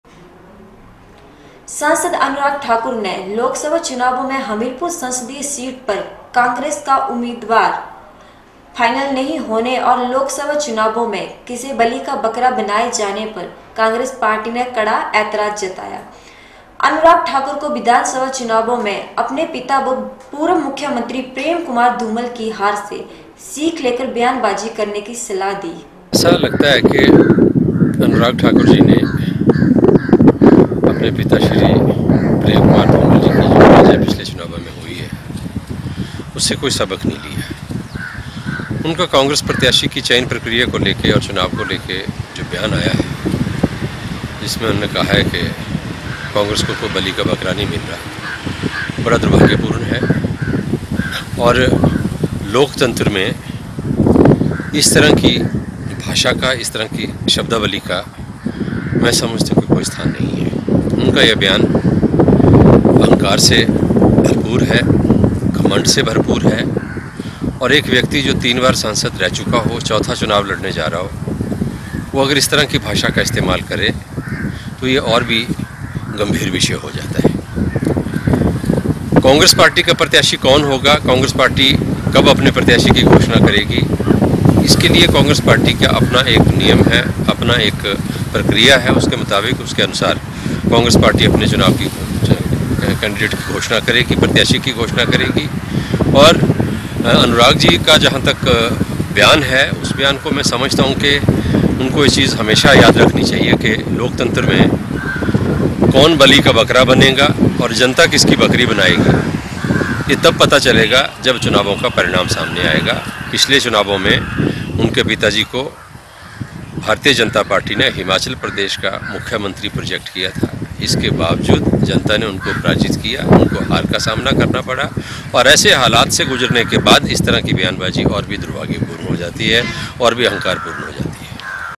Prem Kaushal reversed on the statement of MP Anurag Thakur
सांसद अनुराग ठाकुर ने लोकसभा चुनावों में हमीरपुर संसदीय सीट पर कांग्रेस का उम्मीदवार फानइल नहीं होने और लोकसभा चुनावों में किसे बलि का बकरा बनाए जाने पर काग्रेंस पार्टी ने कड़ा ऐजराज जताया है और अनुराग ठाकुर को विधानसभा चुनावों में अपने पिता व पूर्व मुख्यमंत्री प्रेम कुमार धूमल की हार से सीख लेकर व्यानबाजी करने की सलाह दी है ।